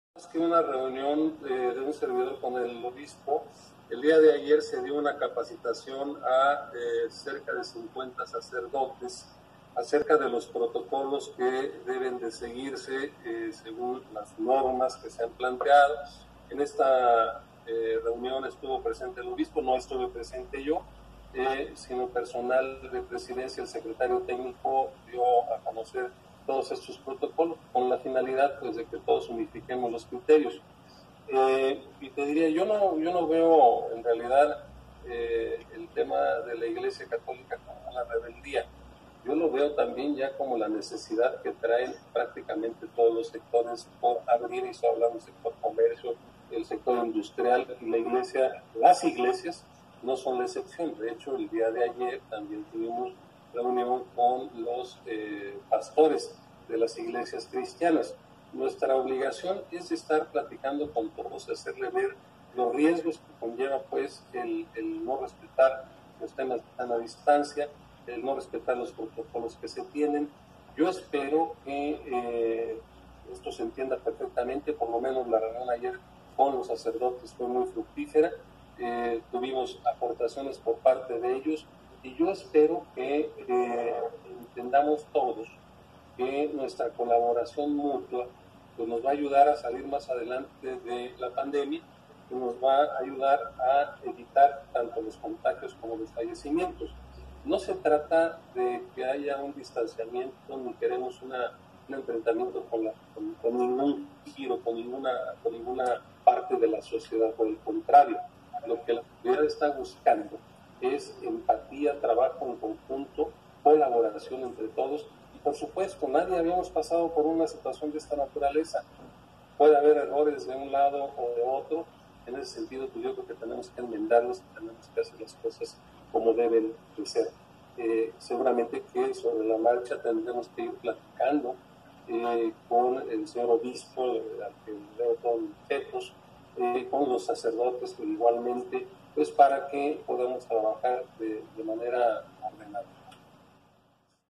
Niega alcalde de Irapuato confrontación con la diócesis
El alcalde de Irapuato Ricardo Ortiz Gutiérrez negó que haya una confrontación con la diócesis de Irapuato por la decisión de mantener cerrados los templos ante incremento de contagios por